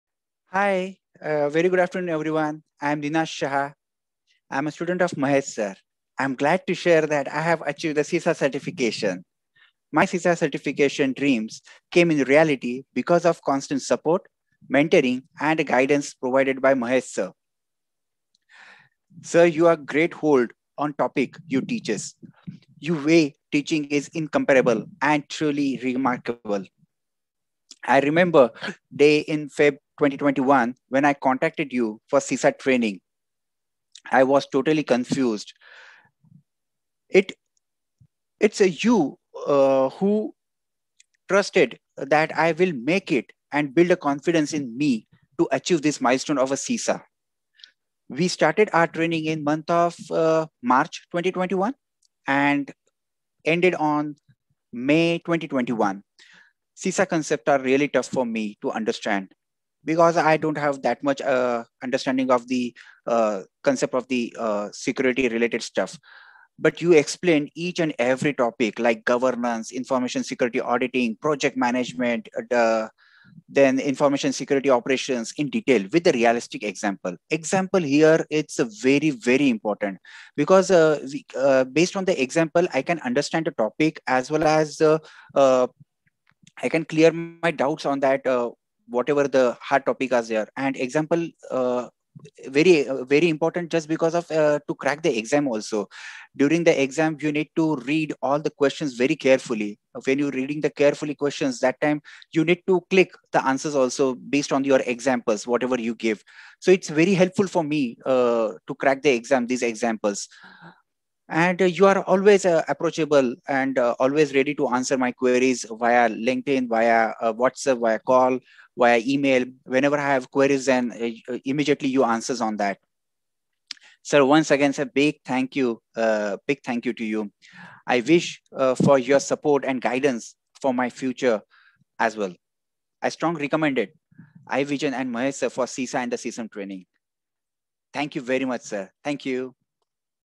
CISA Success Story. Testimonial provided by CISA Certified member of IEVISION, Ninad Shah
We congratulate Mr. Ninad Shah on his accomplishment of CISA Certification.
CISA Training Deliverable:
Thorough CISA all domains preparation with practical examples.
Concept Clearing
Auditing templates
Presentation Material
Courseware
Latest and Revised Mock question sets